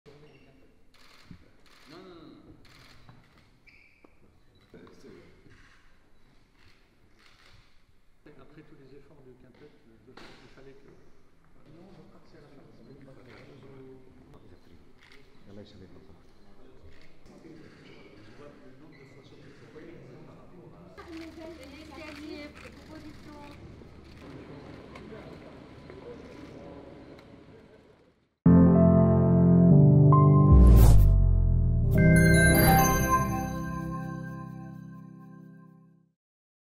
French envoy holds talks with Lebanese PM in Beirut
French special envoy Jean-Yves Le Drian holds talks with Lebanese Prime Minister Najib Mikati during his visit to the Lebanese capital to help resolve divisions that have left the presidency vacant for over a year.